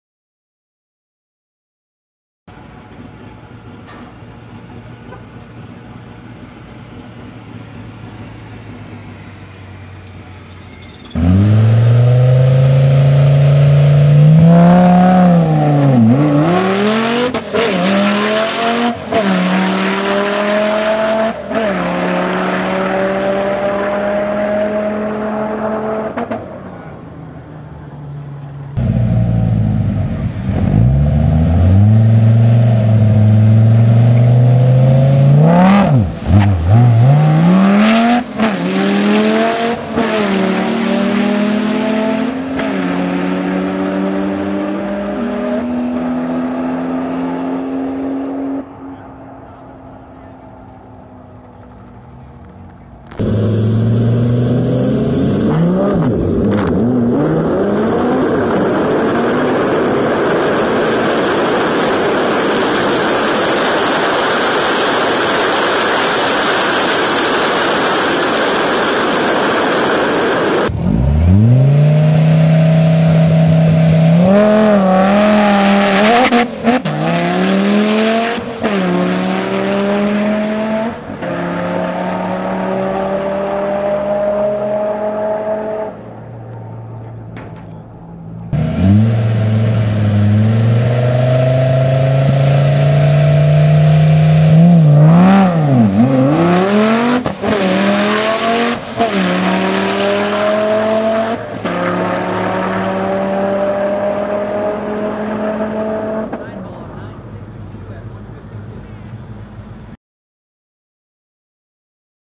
9 Second Supercharged Viper
Tony's supercharged Viper laying down some 9 second passes!
I love watching him real in the white Porsche.